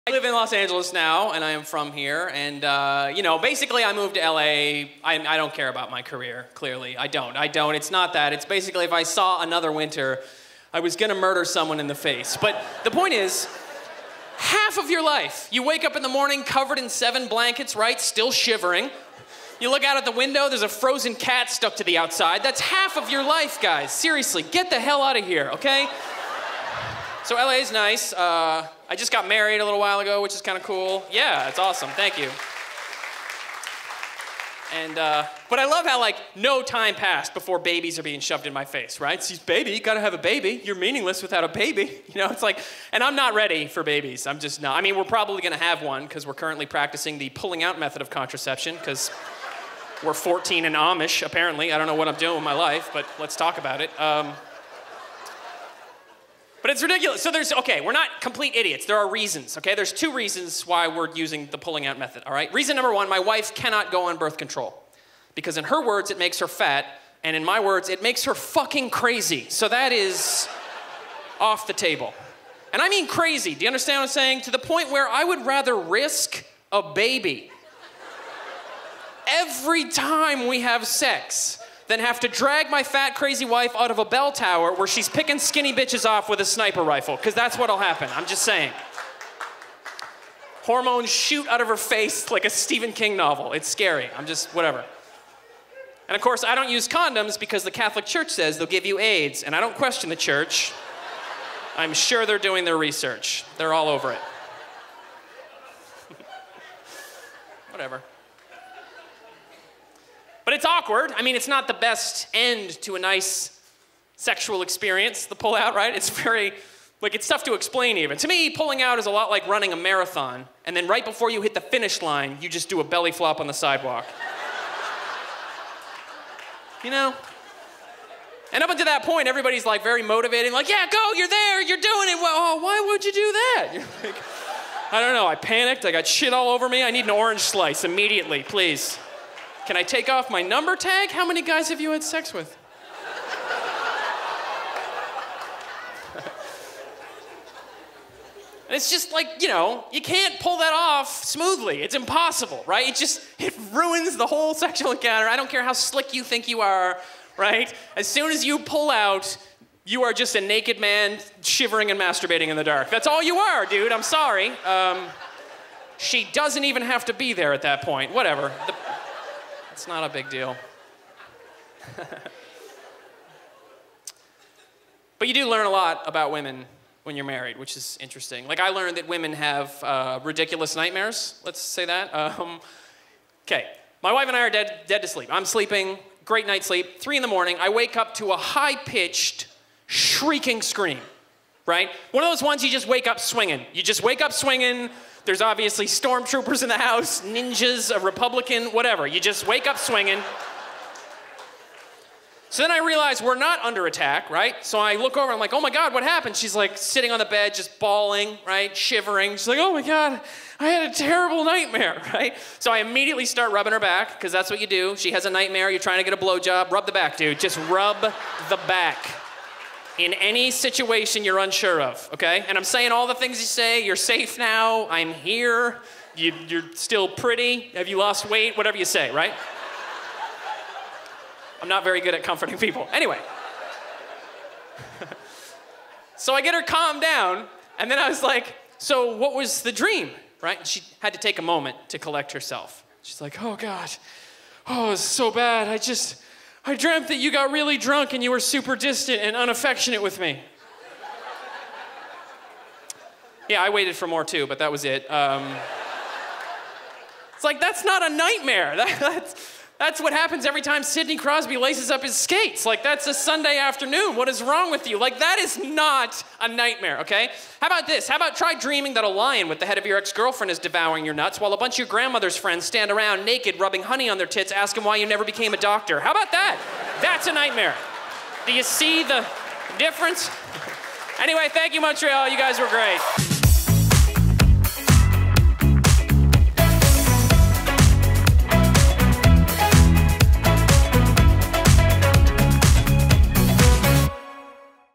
Jay Malone - Pulling out is like Running on the Sidewalk
JayMalone on having a baby, the logistics of pulling out, and crazy nightmares.

JayMaloneStandUp from the JustForLaughs Festival in 2012.